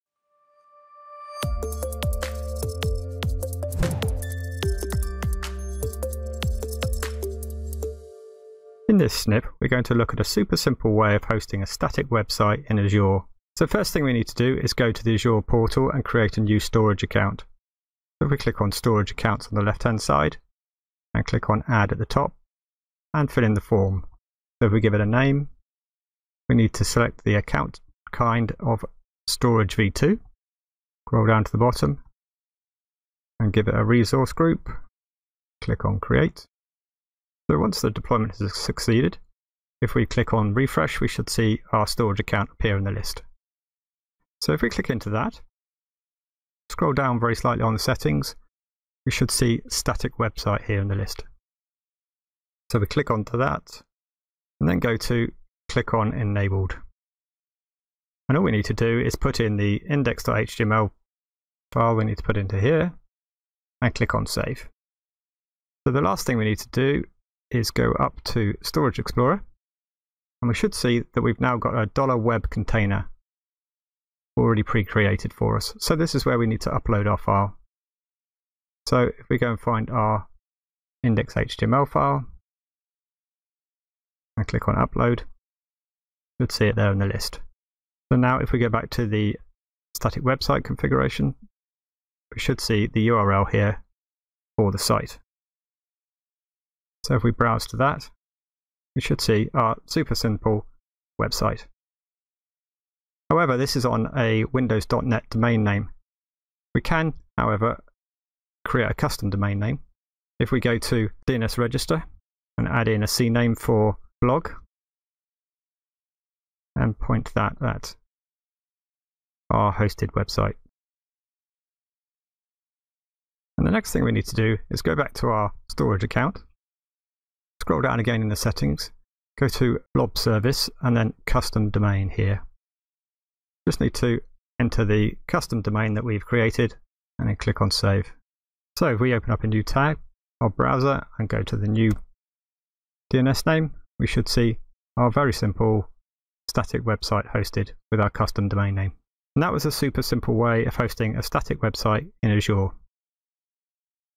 How To Host A Serverless Static Website In Azure
With the increasing popularity of static website generators like Jekyll, full hosting plans seem a bit over to top.  Azure storage accounts are a super simple and easy place to host this type of site.  In this video we demonstrate creating the storage account, enabling static website hosting and applying a custom domain.